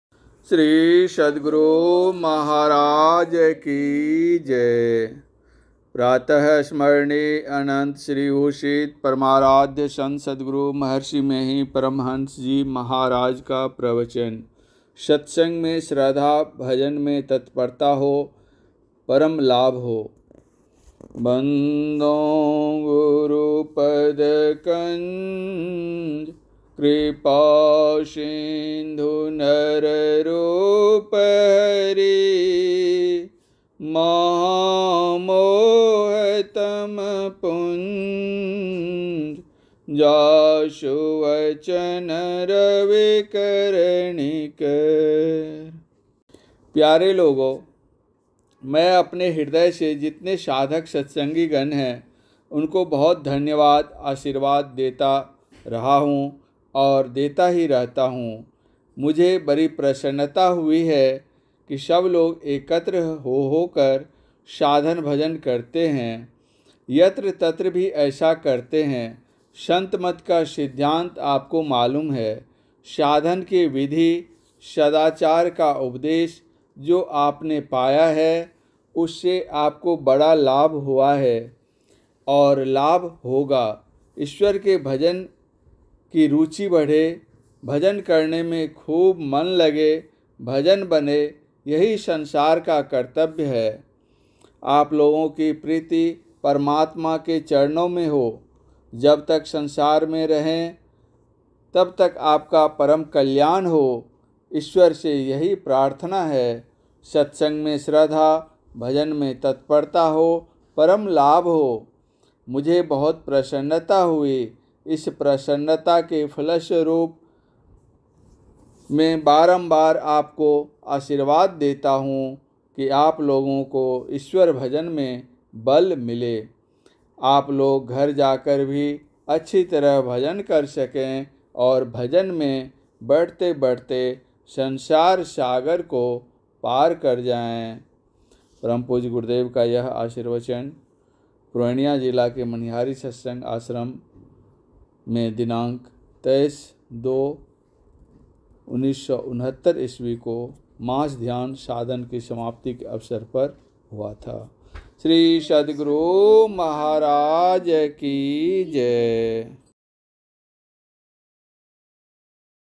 सत्संग में श्रद्धा, भजन में तत्परता हो, परम लाभ हो | संत सद्गुरु महर्षि मेँहीँ परमहंस जी का प्रवचन
सत्संग में श्रद्धा, भजन में तत्परता हो, परम लाभ हो।
संत सद्गुरु महर्षि मेँहीँ परमहंस जी महाराज का प्रवचन | Maharshi Mehi Pravachan | Santmat Satsang Pravachan |

By S.K.Mehta, Gurugram